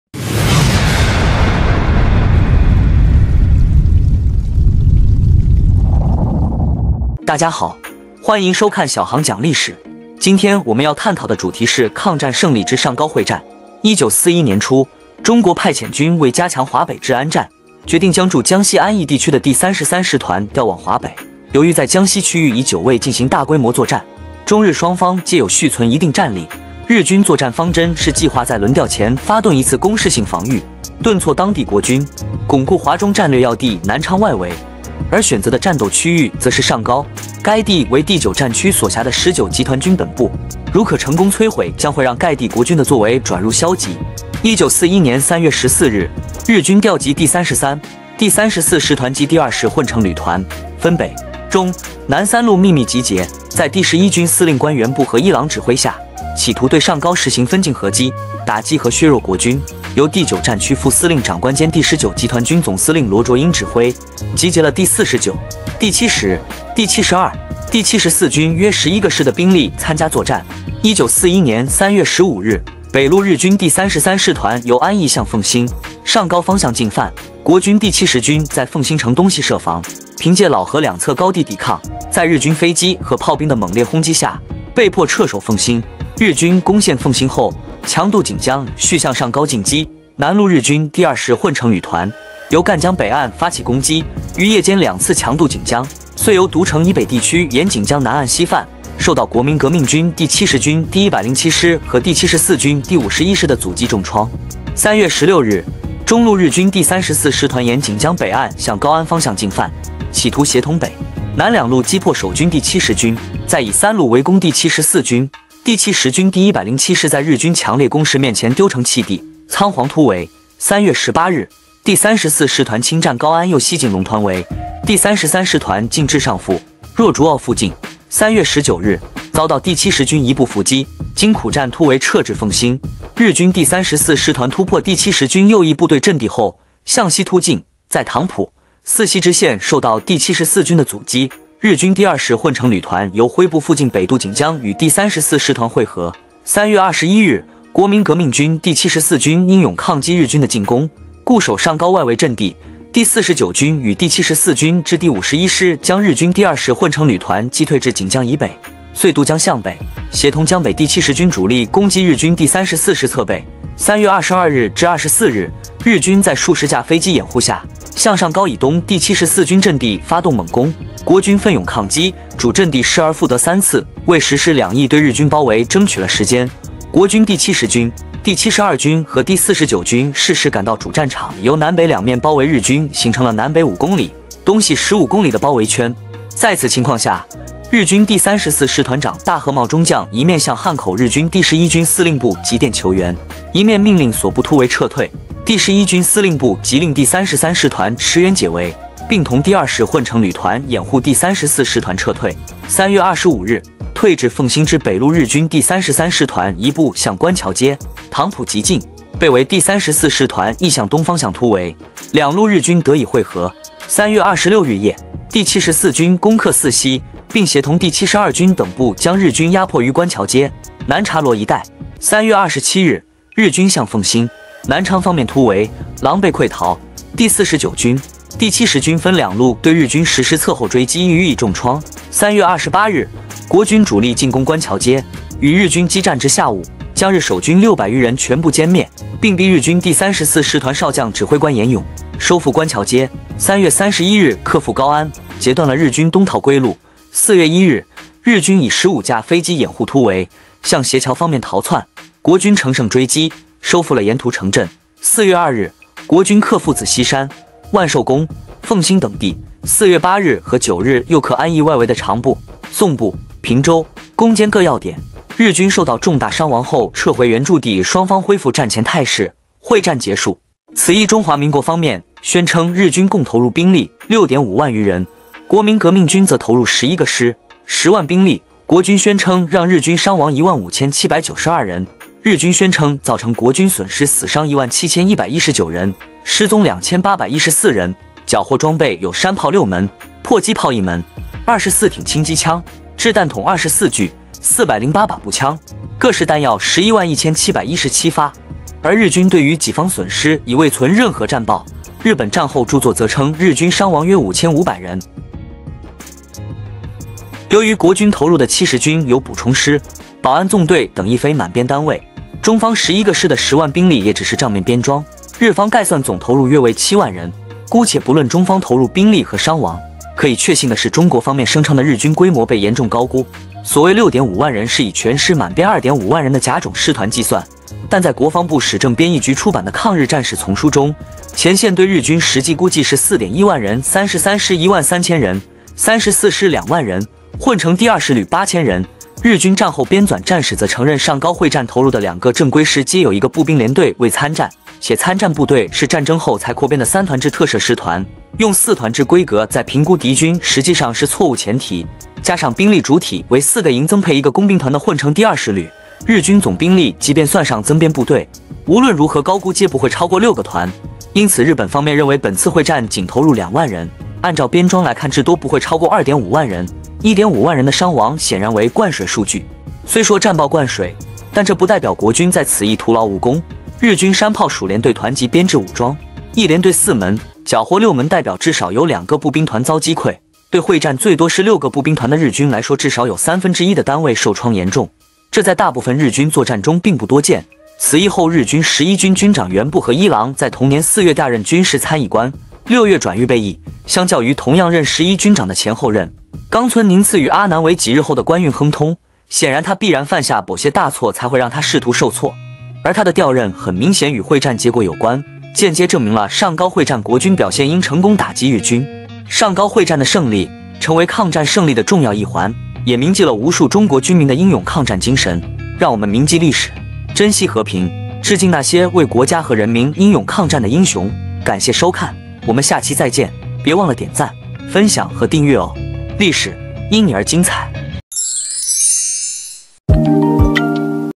小航讲历史：上高会战/Xiaohang talks about history: The Battle of Shanggao/历史/抗战/真相/History/Anti-Japanese War/
1941年初，日军为巩固南昌外围阵地、保证占领区的安全，决定对中华民国国民革命军第9战区部队实施军事打击。这便是著名的上高会战。
日军将此次战事称为“锦江作战”，视为短期战术攻势。然而，国民革命军并非等闲之辈，他们在上高地区做好了充分的准备，准备迎击敌人的进攻。
经过激烈的战斗，国民革命军以出色的战斗素质和坚韧不拔的意志，成功抵御了日军的进攻。
最终，日军在上高会战中遭受了严重的损失，远超出他们的预期。这场战役的胜利，不仅增强了中国军队的信心，也为抗战局势带来了积极的变化。
In early 1941, in order to consolidate the outer positions of Nanchang and ensure the security of the occupied area, the Japanese army decided to launch a military attack on the 9th Theater Unit of the National Revolutionary Army of the Republic of China. This is the famous Shanggao Battle.
The Japanese army called this battle "Operation Jinjiang" and regarded it as a short-term tactical offensive. However, the National Revolutionary Army was not idle. They were fully prepared in the Shanggao area and prepared to meet the enemy's attack.
After fierce fighting, the National Revolutionary Army successfully resisted the Japanese attack with its excellent fighting qualities and perseverance.
In the end, the Japanese army suffered serious losses in the Battle of Shanggao, far exceeding their expectations. The victory of this battle not only enhanced the confidence of the Chinese army, but also brought positive changes to the situation of the Anti-Japanese War.